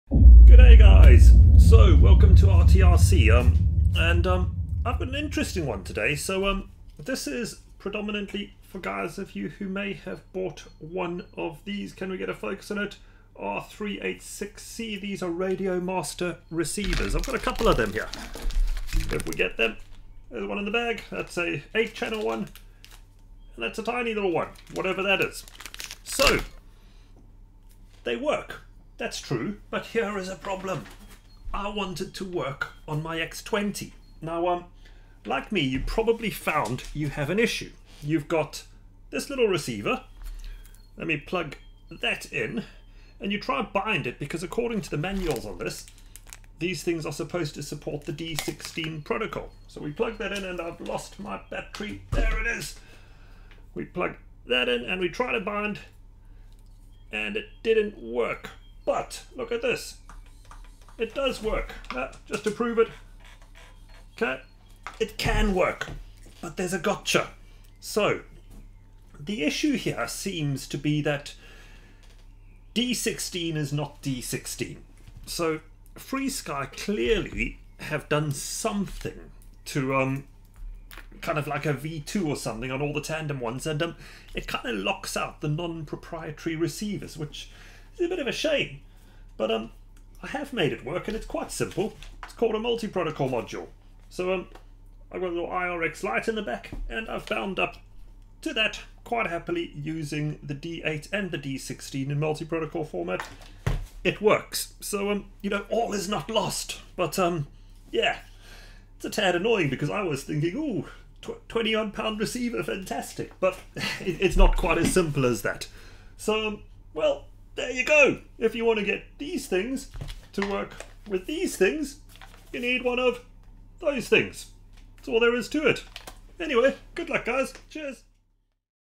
RADIOMASTER RECEIVERS VS FRSKY TANDEM RADIOS
So..  can you make the radiomaster multiprotocol receivers bind to the X20.  Yes...  but how?

Happy flying...

Looking to improve your youtube channel performance?